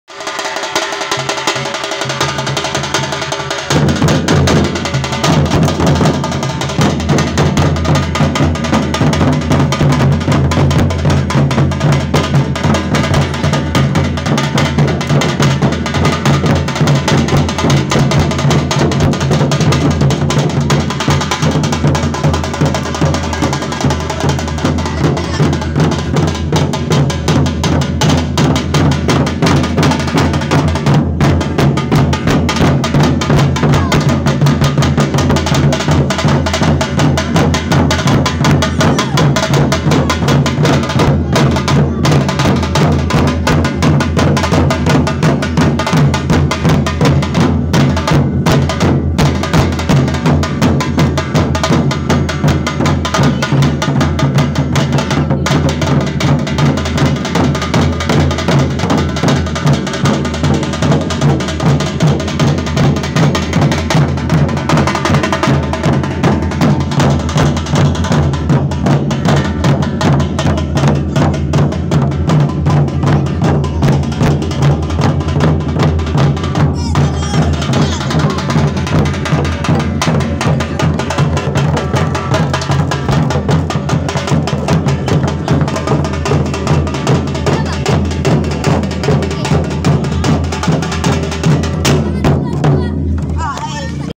Gendang Pasa SDN 11 PAUH BARAT
Pelajaran Ekstrakukuler Gendang Tasa SDN 11 PAUH BARAT KOTA PARIAMAN